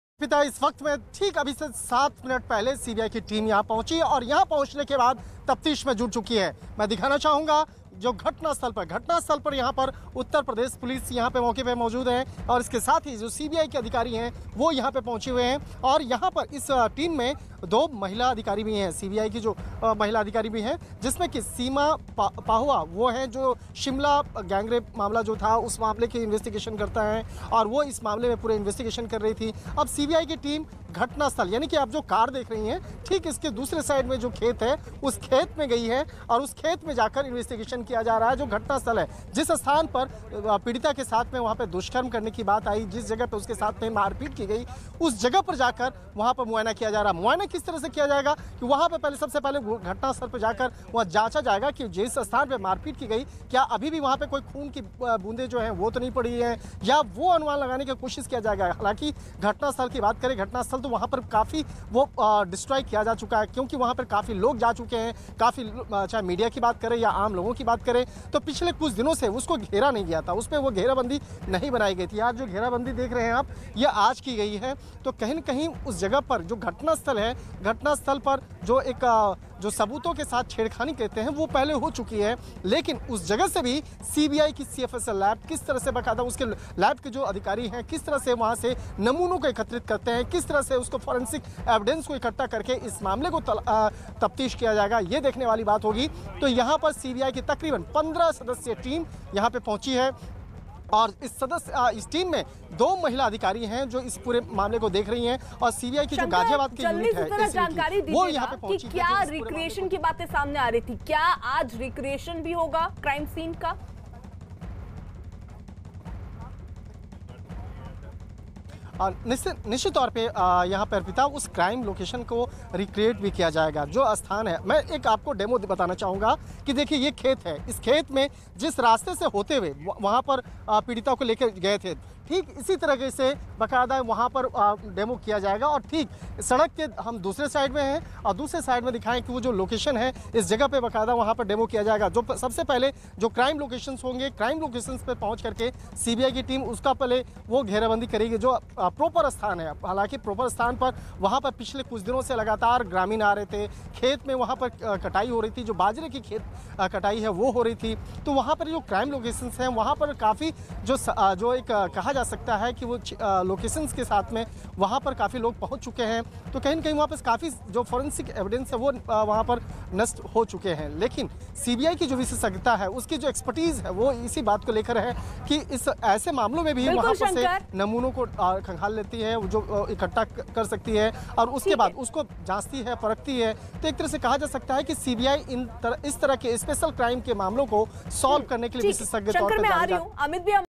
Hathras Case: वारदात की जगह पहुंची फॉरेंसिक टीम, 15 सदस्यों की टीम स्पॉट पर रीक्रिएट करेगी पूरा सीन
CBI की टीम ने हाथरस कांड की छानबीन की औपचारिक शुरुआत कर दी है. टीम में शामिल अधिकारी मंगलवार को क्राइम स्‍पॉट पर पहुंचे. पीड़‍िता के भाई को भी घटनास्‍थल पर बुलाया गया है, ताकि घटना के बारे में पूरी जानकारी जुटाई जा सके. हाथरस कांड में गंभीर रूप से घायल युवती की इलाज के दौरान दिल्‍ली के सफदरजंग अस्‍पताल में मौत हो गई थी. इसके बाद इस मामले ने तूल पकड़ लिया था. बाद में उत्‍तर प्रदेश सरकार ने मामले की जांच सीबीआई को सौंप दी थी.